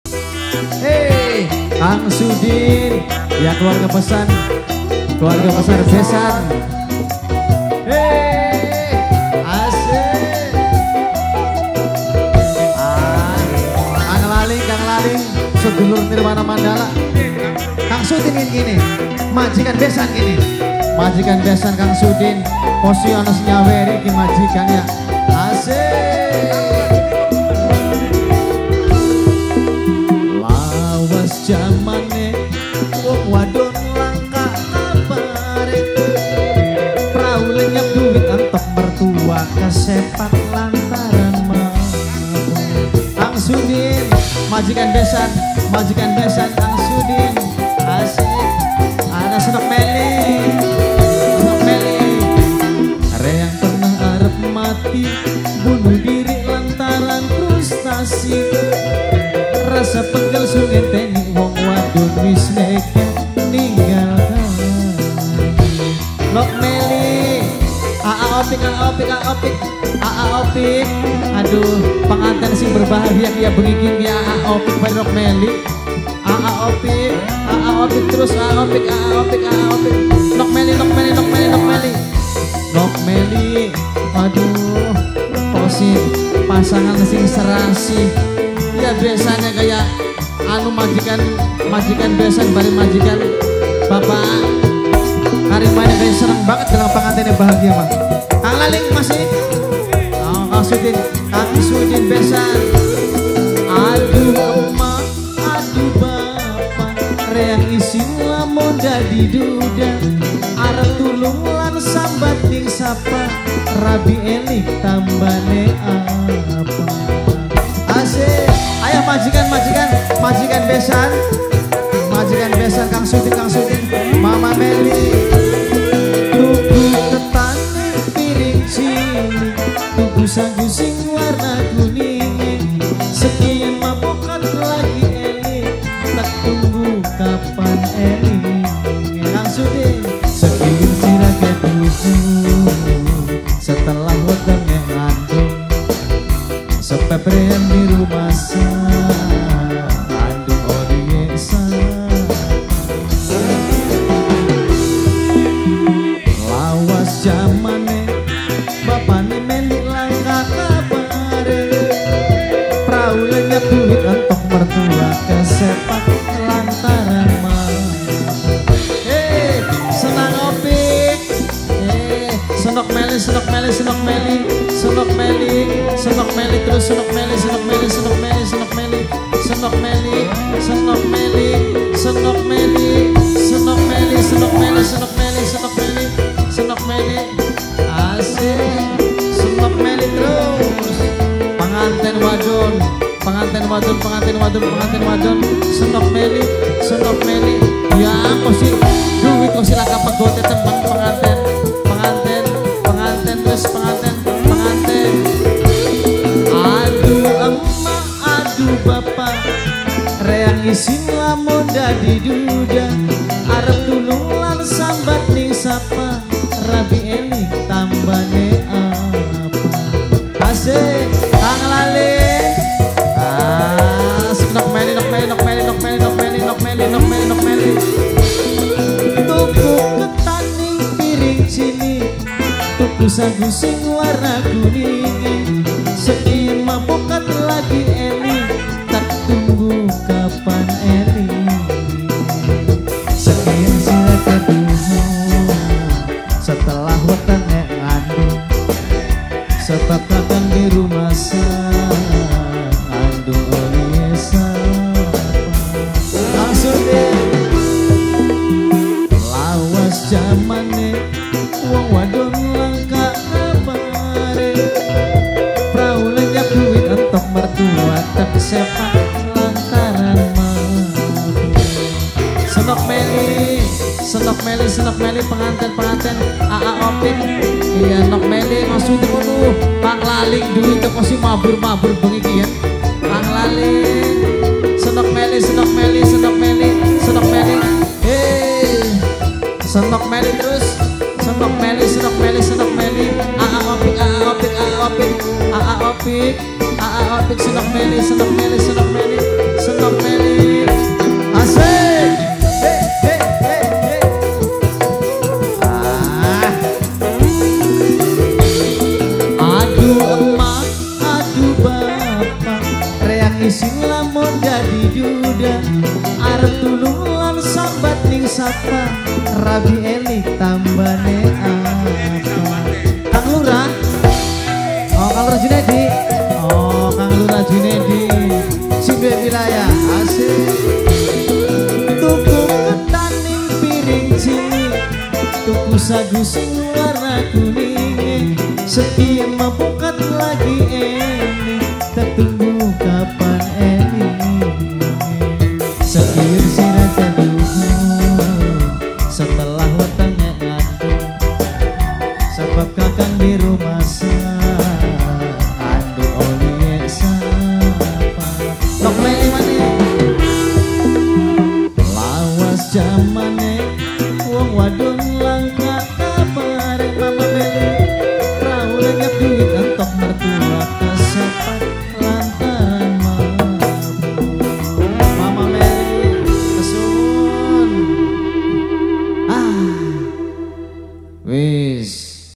RABI ELIK SUKA WIJAYA SHOW NIRWANA MANDALA SAKTI PESTA HAJAT BPK KARIMAN IBU ROPIKOH BUGISTUA
PEMANGKU HAJAT :
BPK KARIMAN / IBU ROPIKOH

PELAKSANAAN
18 OKTOBER 2023

TEMPAT
DESA BUGIS TUA
KEC. ANJATAN  KAB.INDRAMAYU